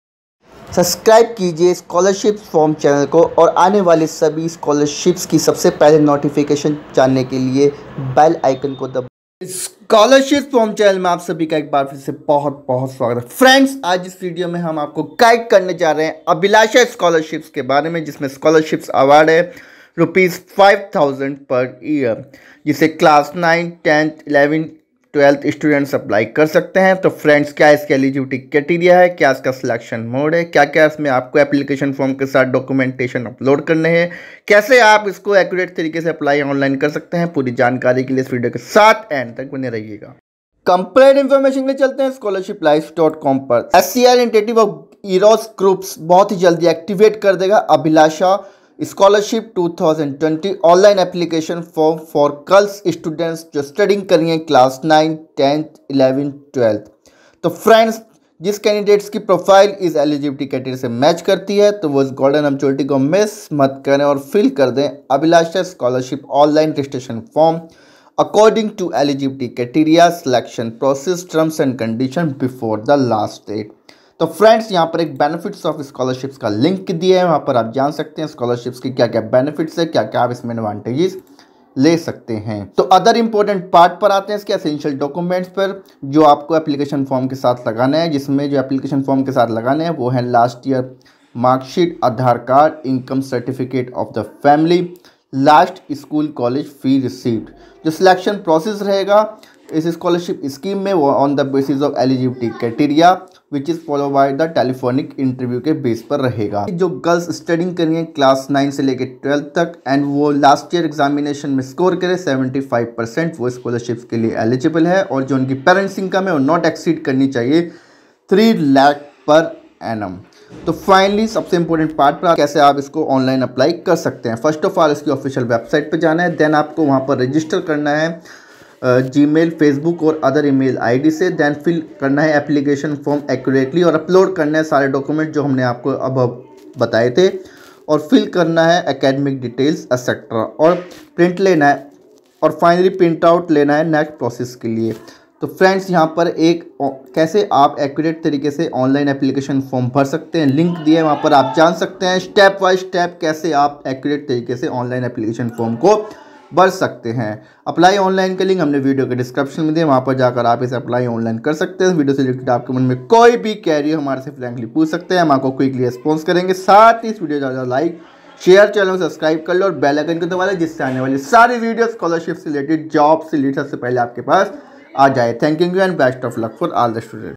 Abhilasha Scholarship 2020 Application form. Eligibility, Selection
CSR initiative of EROS Group  will soon activate the  Abhilasha Scholarship Application Form 2020 for a girl students (Class 9 to 12th)